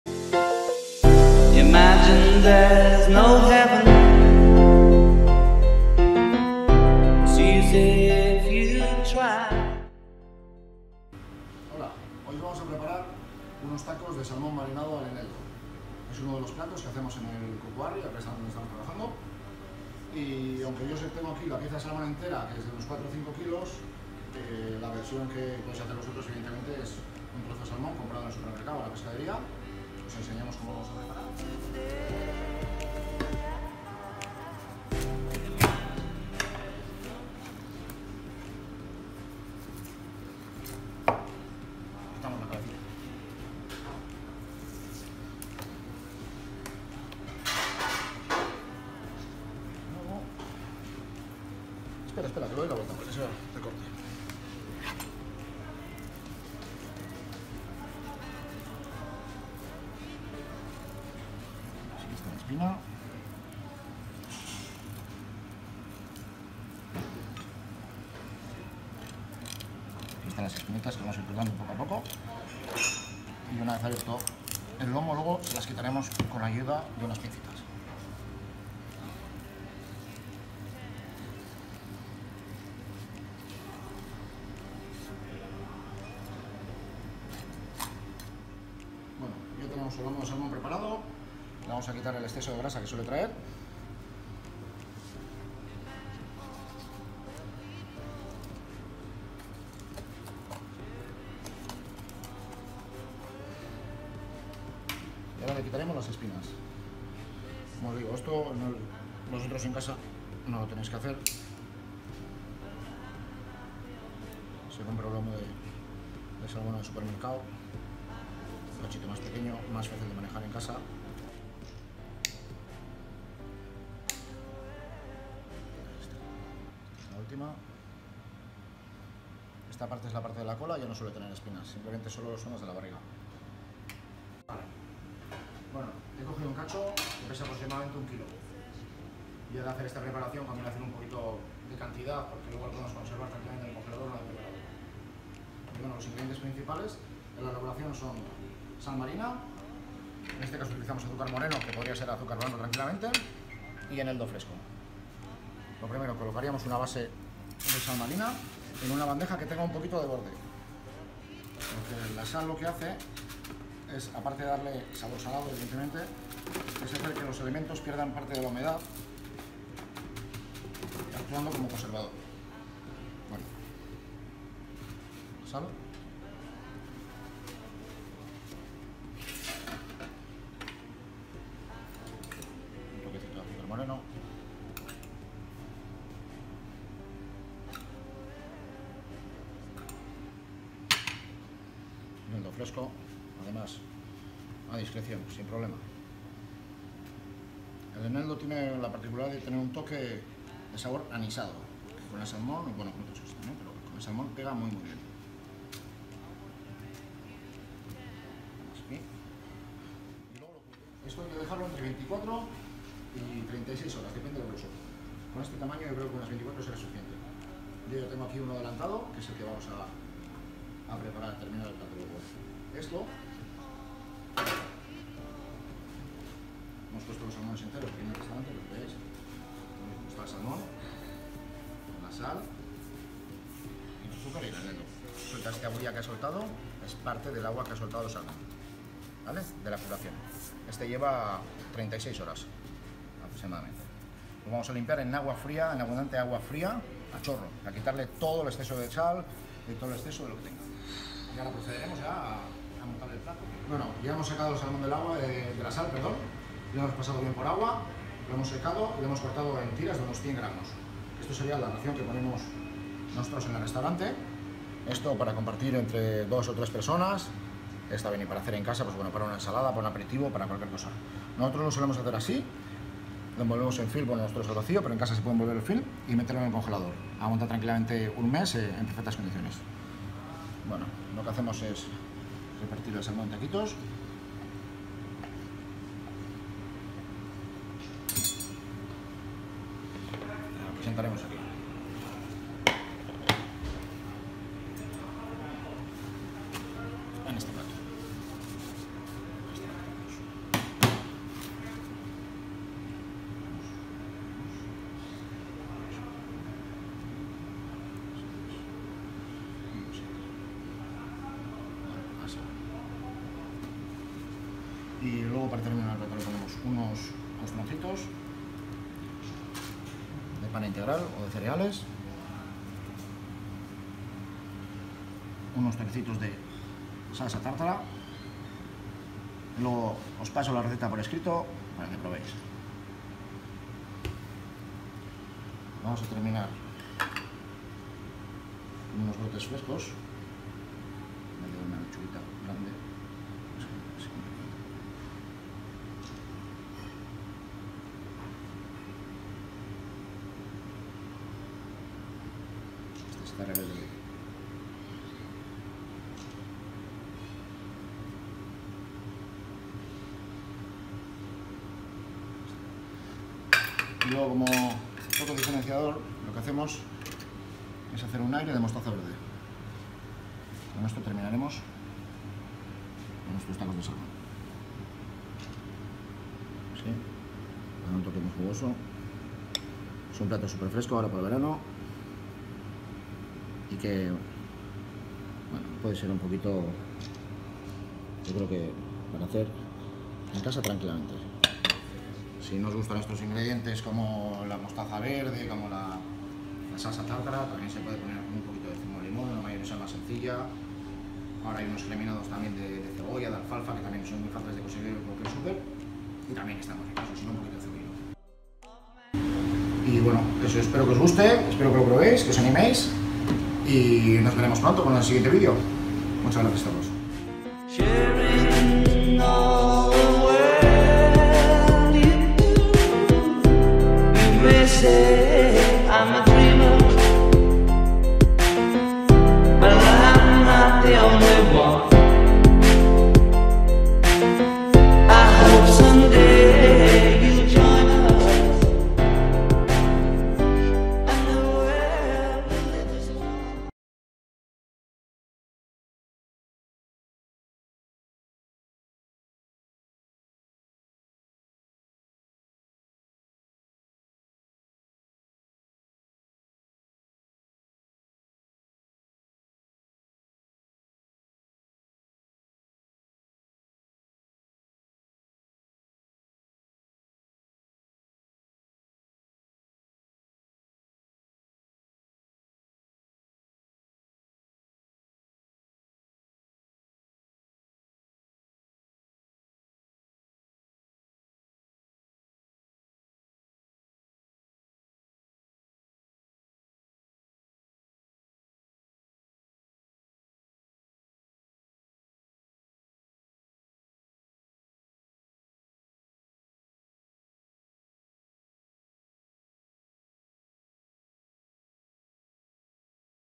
Tacos de salmón marinado (paso a paso)| Chef Martín Aristondo | Receta
Tacos o dados de salmón marinados al eneldo con salsa tártara: receta fresca y apetecible. Ver también artículo y transcripción "Dados de salmón marinados: receta fresca y apetecible ", del Chef Martín Aristondo · Restaurante Kukuarri · San Sebastian, en el blog SoplosViajeros · pequeños mundos con duende

VÍDEO RECETA: Tacos de salmón marinado de Chef Martín Aristondo (Restaurante Kukuarri).

1 kg de salmón fresco
Sal marina gorda
Azúcar moreno
Eneldo fresco

SALSA TÁRTARA
Mayonesa
Alcaparras
Pepinillos en vinagre
Perejil

AIRE DE MOSTAZA
Agua
Salsa de soja
Mostaza verde
Lecitina se soja